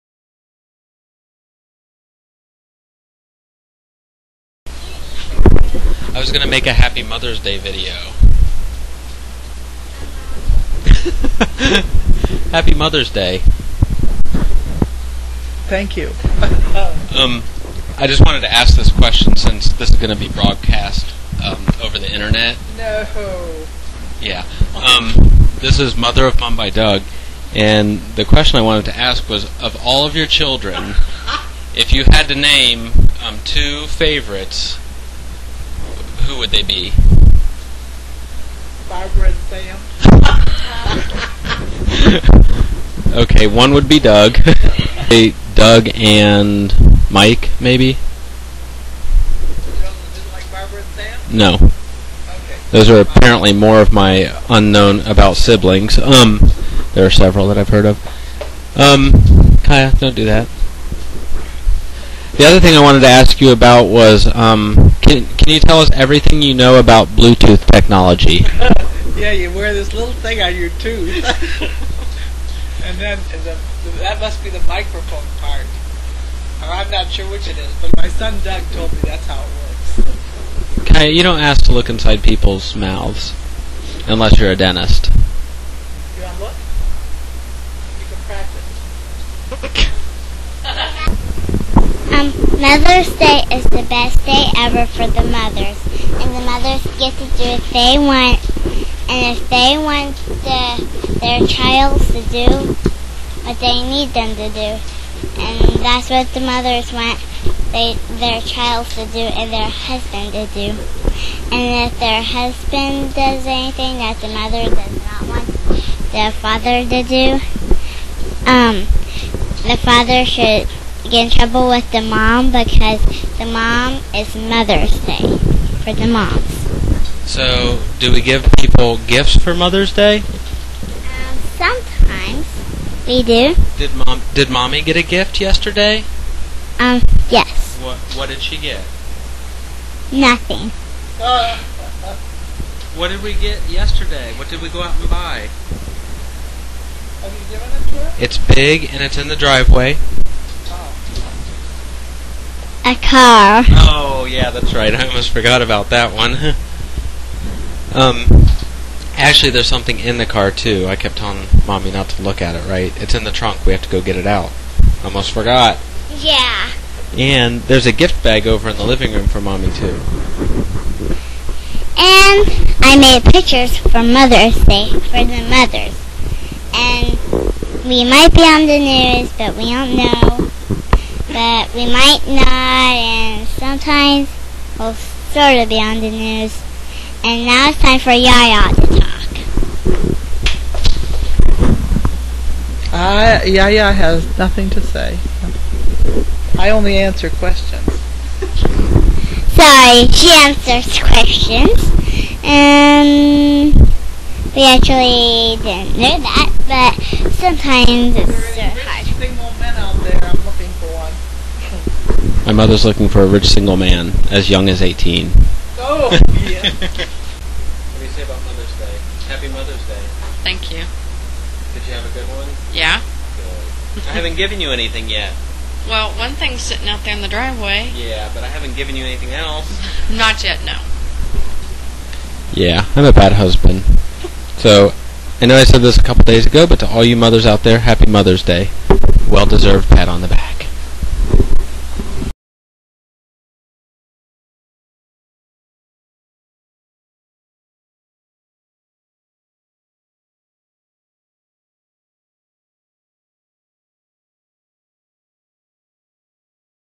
Happy Mother's Day!
Just a video of my mom, my wife, and my daughter on Mother's day.